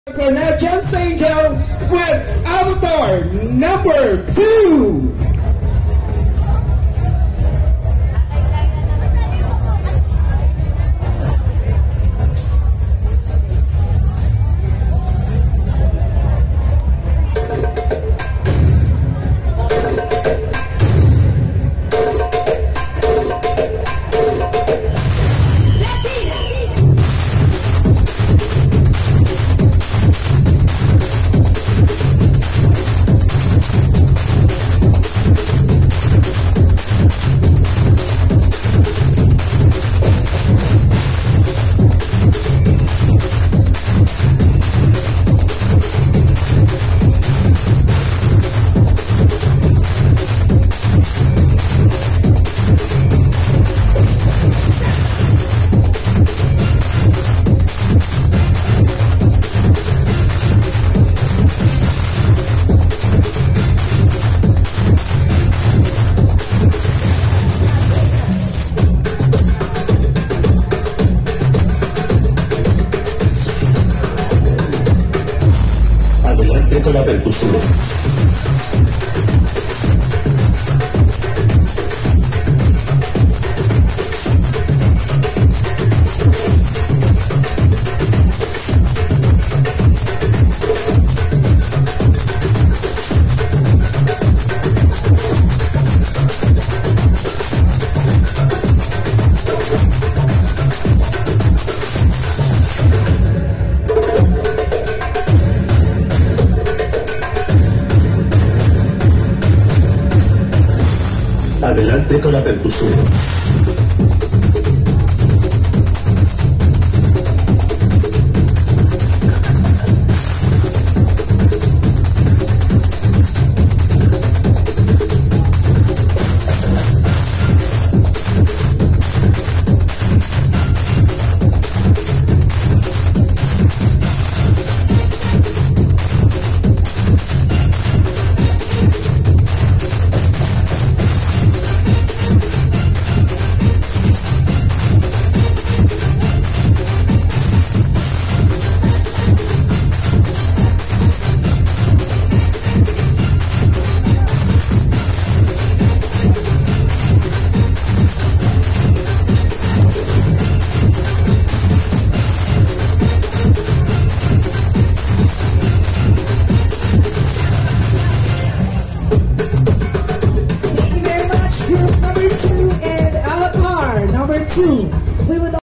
Bacolod Mardigras 2023 "Candidate no. 2" | Bacolod City Public Plaza
Bacolod Mardigras 2023 "Candidate no. 1" | Bacolod City Public Plaza - Masskara Festival Event Held in Bacolod Public Plaza